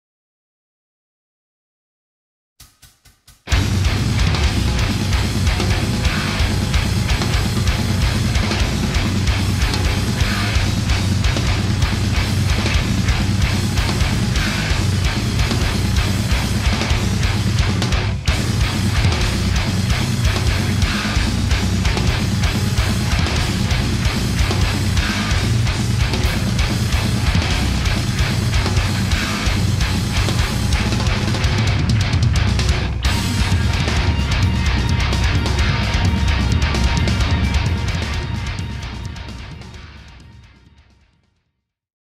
Rotten Gunshot 2
Time Lapse fotografico di un Effetto Speciale creato da MarshallMUA